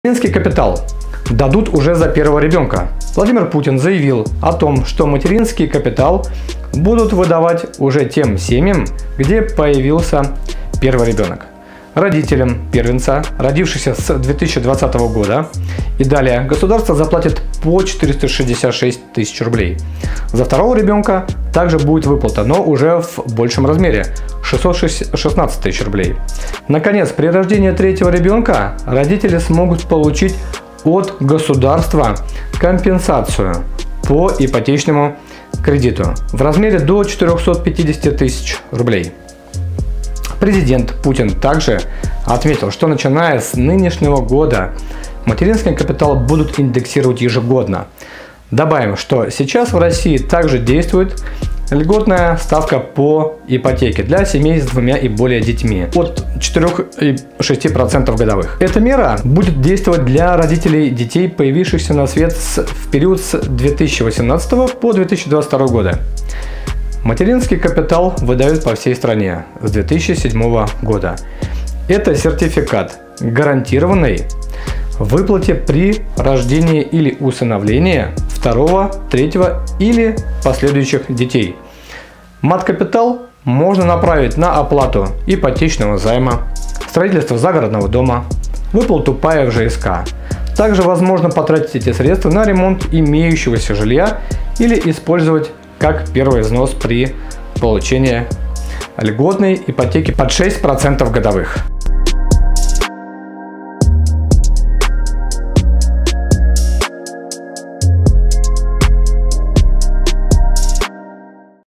Материнский капитал 2020. За первого ребенка.
Материнский капитал 2020. Теперь дадут за первого ребенка //+16
в этом видео Вы узнаете:
👉 Владимир Путин заявил о том, что материнский капитал будут выдавать уже тем семьям, где появляется первый ребенок
►Родителям первенцев, родившихся с 2020 года и далее, государство заплатит по 466,6 тысячи рублей. За второго ребенка также будет выплата, но уже в большем размере (616,6 тысячи рублей). Наконец, при рождении третьего ребенка родители смогут получить от государства компенсацию по ипотечному кредиту в размере до 450 тысяч рублей.
►в России также действует льготная ставка ипотеки для семей с двумя и более детьми в 4,5-6% годовых. Эта мера будет действовать для родителей детей, появившихся на свет в период с 2018 по 2022 годы.